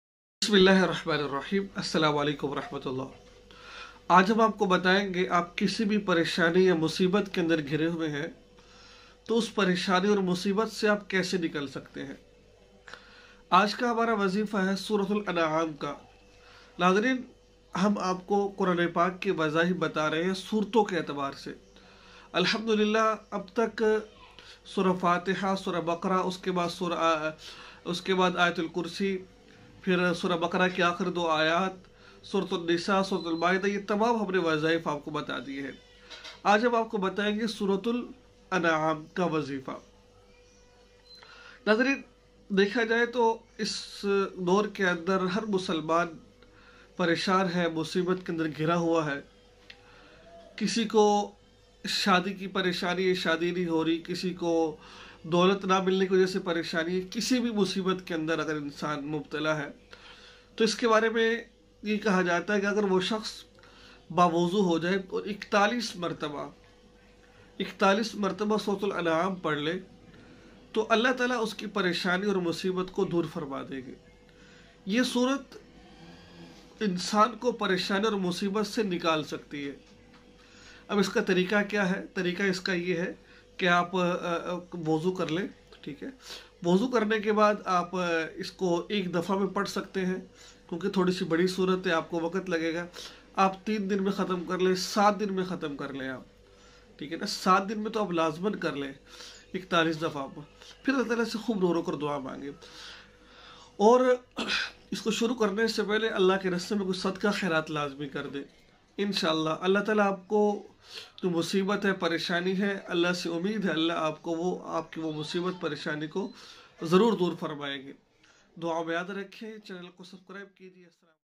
Surah Inam Ka Wazifa ! sakht mushkil ka wazifa ! sakht pareshani ke waqt ki dua ! Molana Masood Nqb
Aaj ki is video main hum ap ko batye ge ke sakht pareshani ke waqt ki dua Aur Wazifa
Wabsaid

youtoub1

Telgram

Facbook

Tawetr

Istgram

Whatsap

 Skipe